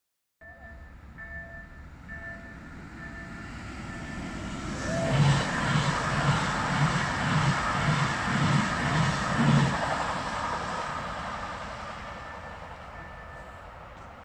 POLSKIE PENDOLINO ED161 , czyli szrot na torach
Polska myśl techniczna, ED161 okrzyknięte przez media Polskim Pendolino, które okazało się wielką wydmuszką :D